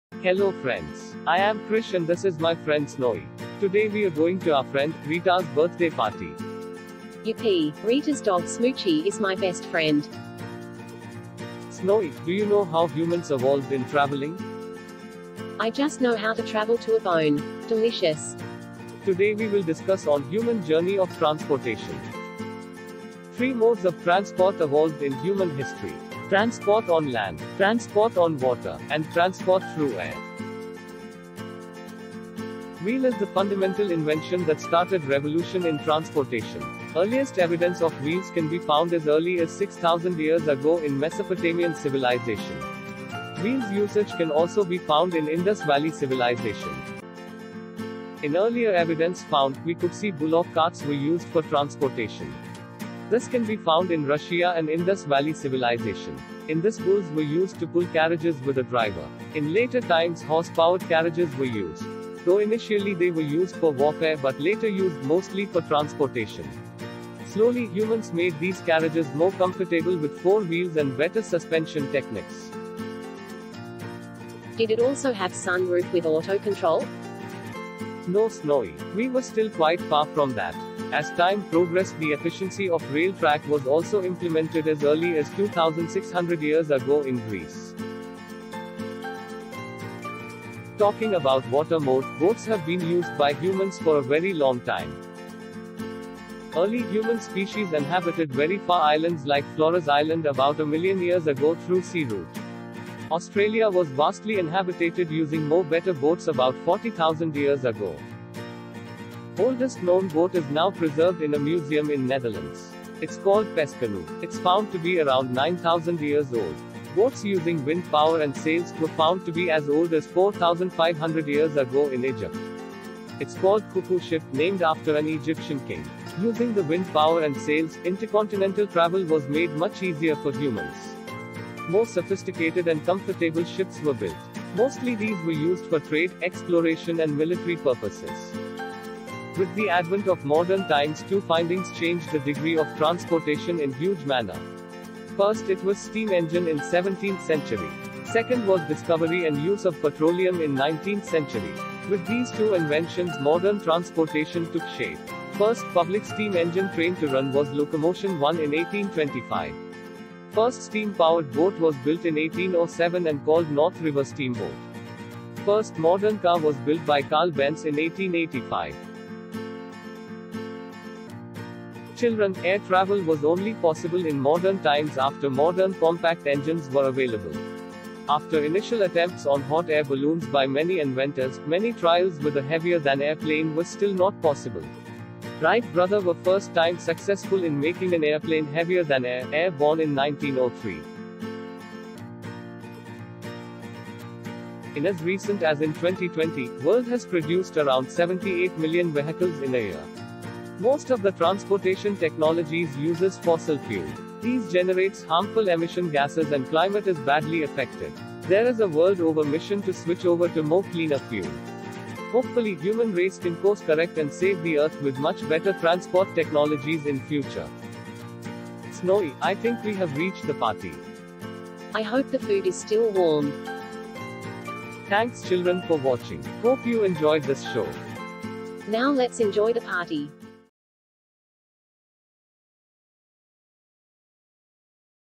Origin of Transport| Invention of Transport | Krish & Snowy Show| ShishuWorld Kids
Krish and Snowy talk about invention of Transport in this video. How and when were the transport started by humans? What are different kinds of transport?

Videos for school kids| School for kids | Best kids videos | Learning Videos